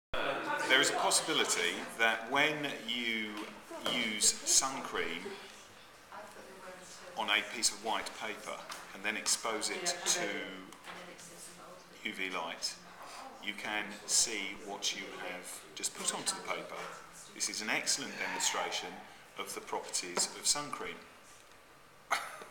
Sun Cream 1.AVI
The Physics group at the National Practitioner Network meeting at Think Tank chose to focus on Sunscreen and applying some of the techniques they had developed during the day at Think Tank to create an innovative approach to teaching about Sunscreens in Triple Science GCSEs